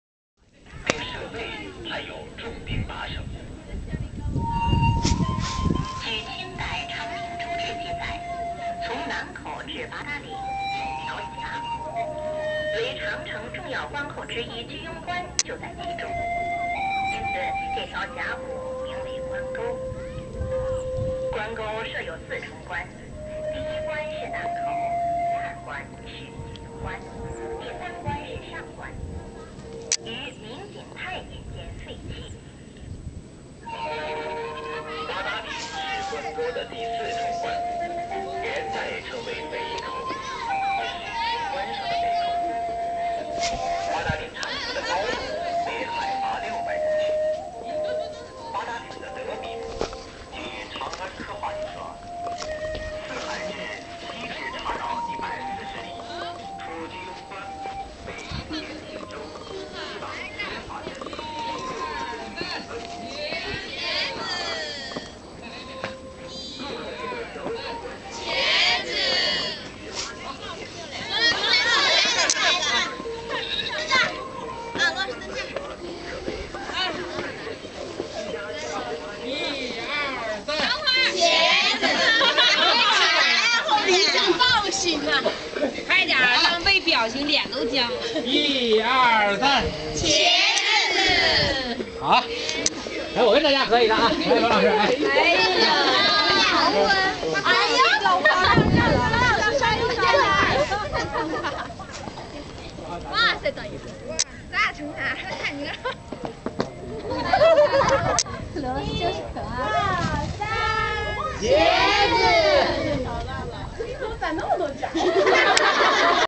Great Wall BaDaLing, Beijing; Große Mauer, Peking, China
The most famous part of the Great Wall in China.

➡ BTC Tip Jar: 1BkHz34jmUsq8PgTkhFVqyF9aM8poqjLt8

💰 Try Coinbase and get $10 in FREE Bitcoin when you buy or sell $100 or more in crypto!